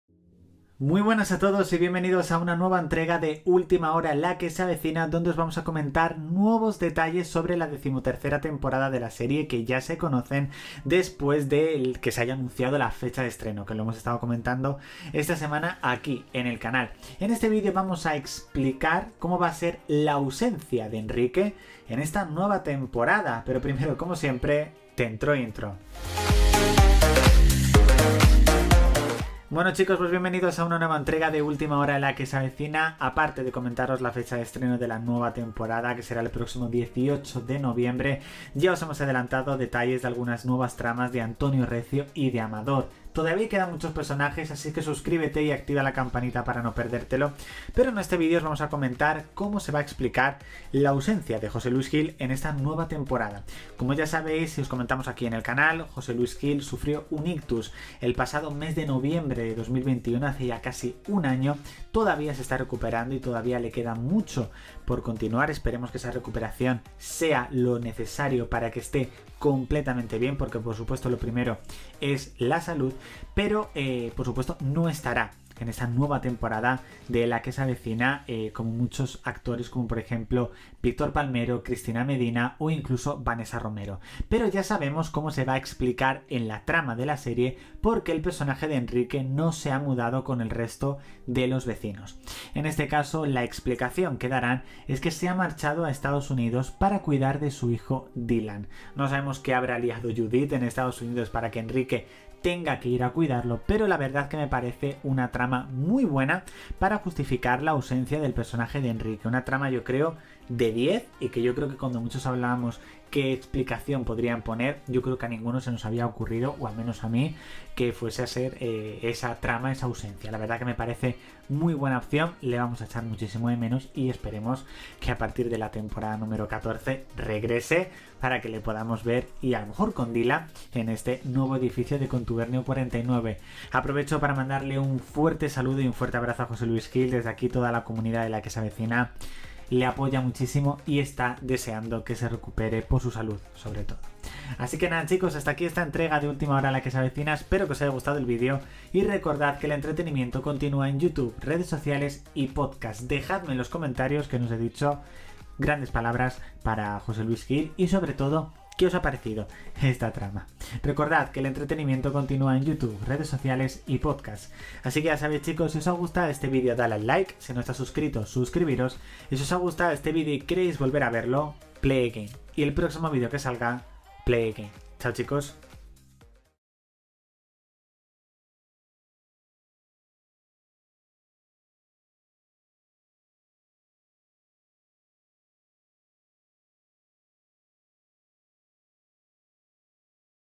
LA QUE SE AVECINA 13ª Temporada 😱 Así será la ausencia de Enrique
▶︎ Descubre cómo justificarán la ausencia de Enrique en la 13ª Temporada de LA QUE SE AVECINA ¿Dónde está? ◀︎